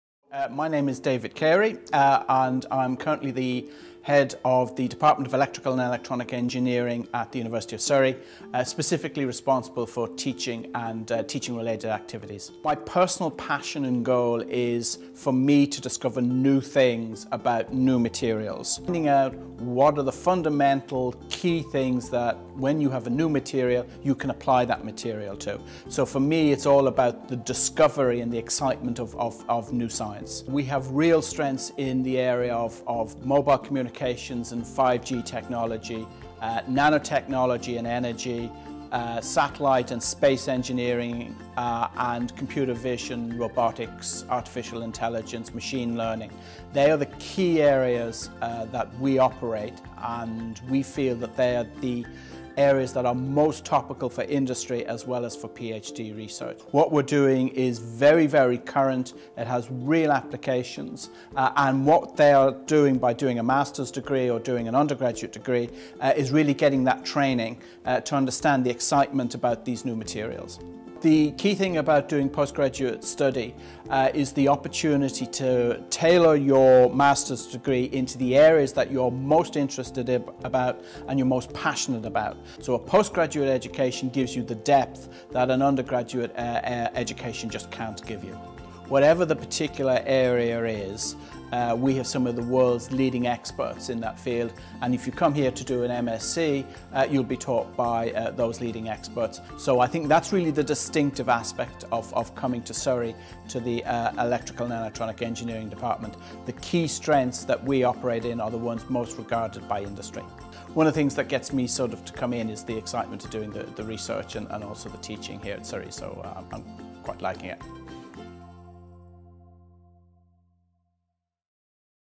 The benefits of a Masters degree in Electrical and Electronic Engineering
Learn how a Masters degree can help you focus your interests and pursue your passion for a career in Electrical and Electronic Engineering.

Explore Surrey’s degrees in Electrical and Electronic Engineering.